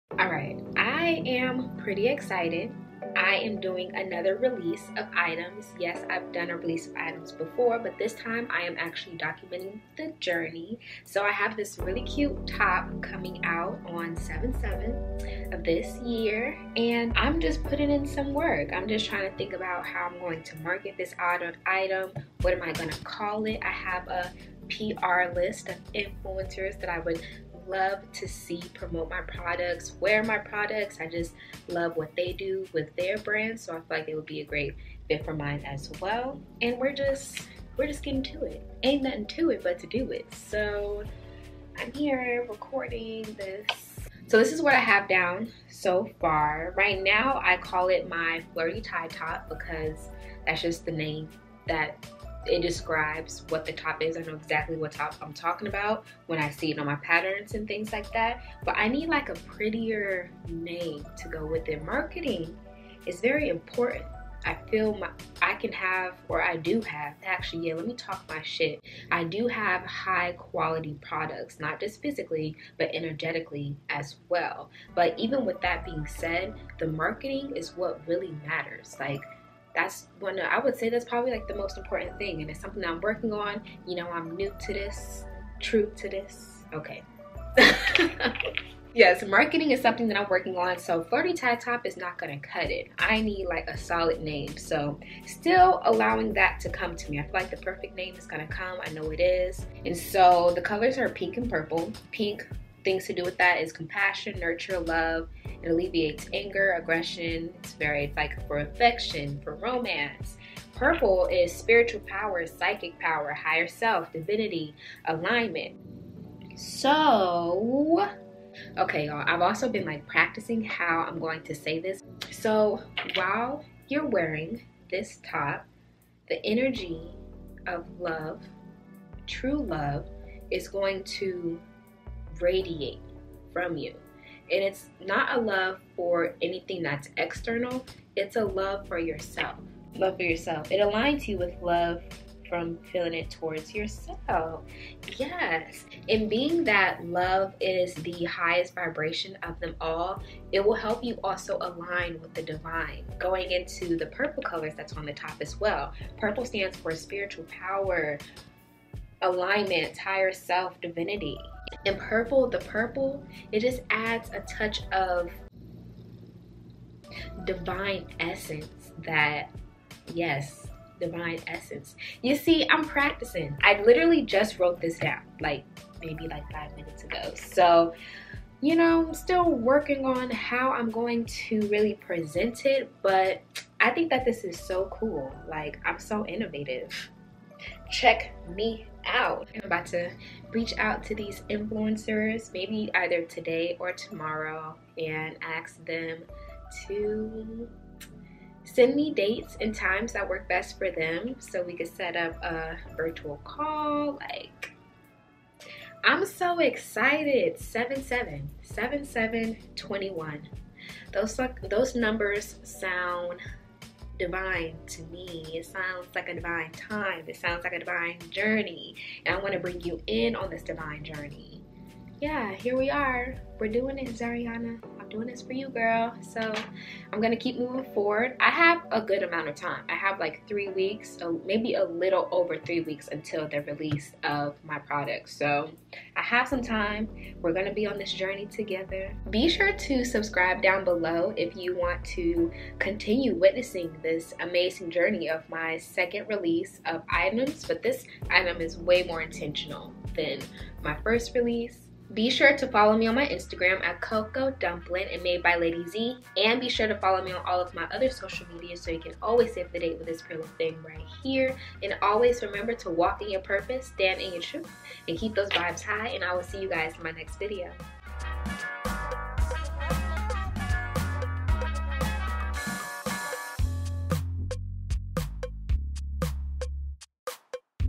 LAUNCHING MY NEW COLLECTION! Journey to my release date 7/7/21
business launch, fashion brand, new business, clothing brand, influencer

Music From:
EpidemicSoundcloud
SanovaBeatz